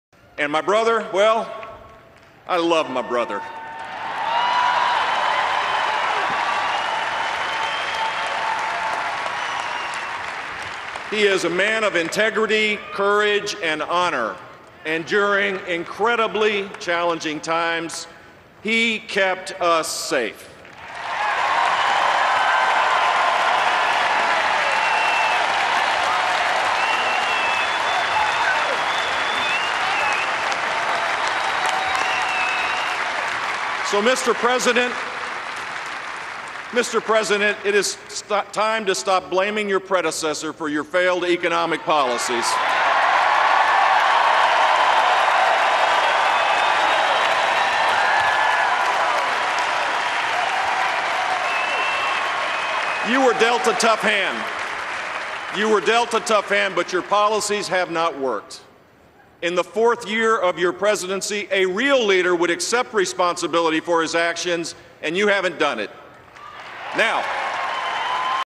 Jeb Bush to Obama: Stop blaming my brother
Former Florida Governor Jeb Bush addressed the Republican National Convention and said that President Barack Obama was dealt a tough hand, but his policies have not worked to improve the U.S. Economy. (Aug. 30)